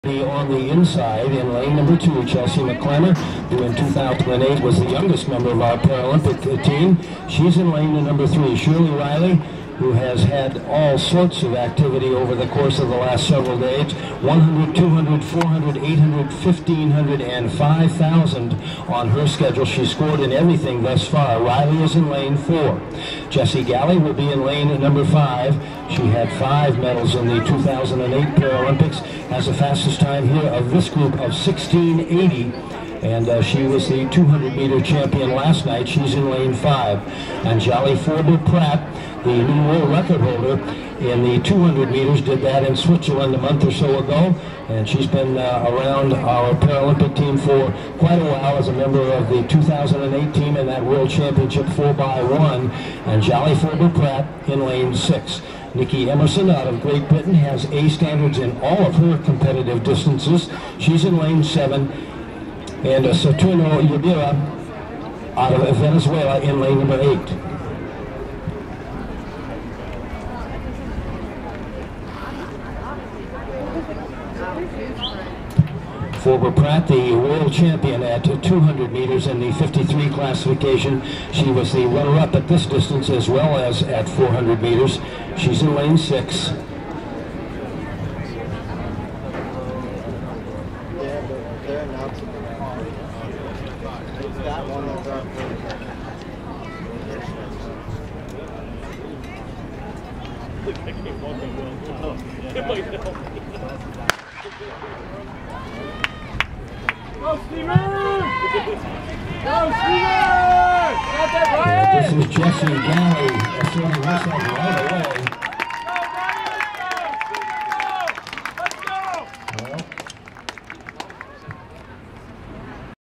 Men 100 M T53 - 2012 Paralympic Trials
1 Bushell, Michael        14.38
2 Siemann, Brian          14.83
3 Abbott, Zachary         15.01
4 Jesus, Aguilar            15.72
5 Hunt, Robert              15.98
6 Binning, Stephen       16.02
7 Torres, Arturo            16.29
8 Cuevas, Jim              16.81